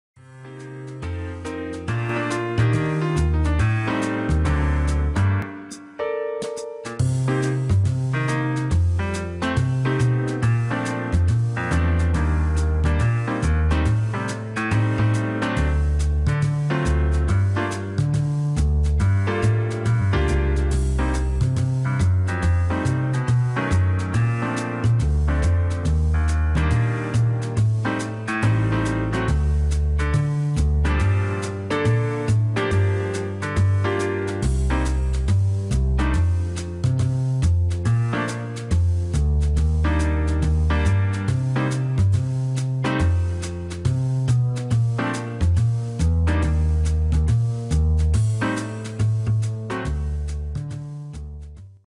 Optio430と西に向かって（倉敷）美観地区
倉敷川沿いをぶらぶら・・・